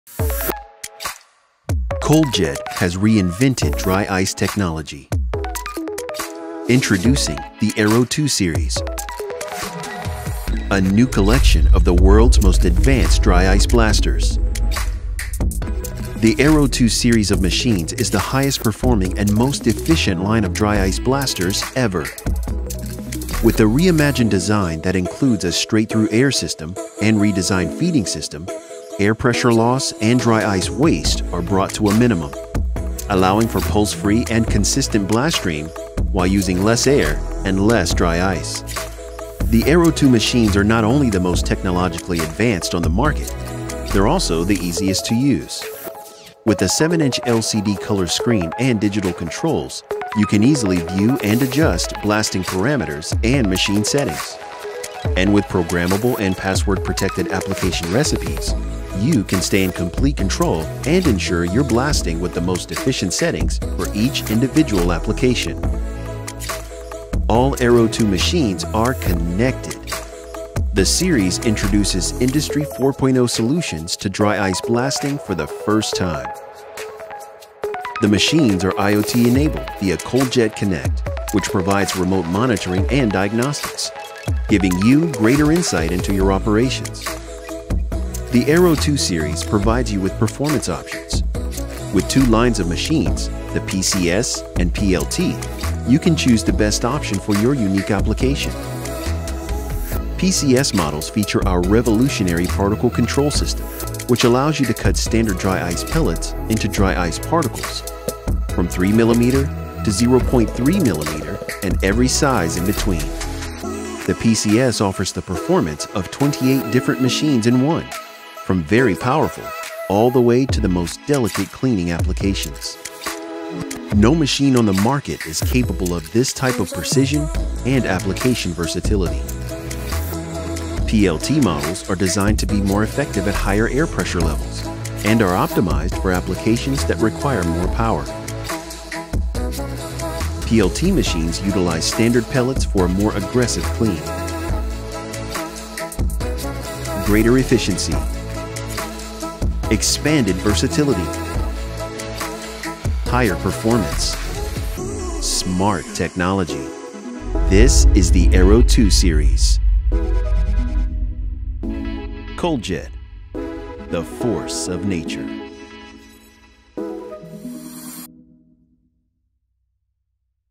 The Cold Jet Aero2 Series
The Aero2 Series is the highest performing line of dry ice blasters available. They are truly smart machines that give you complete control.

The Aero2 line puts you in complete control. Fine tune the blasting parameters for your unique application, while using less air and less dry ice. The machines are easy to use with an intuitive HMI display and are ergonomically designed for your comfort.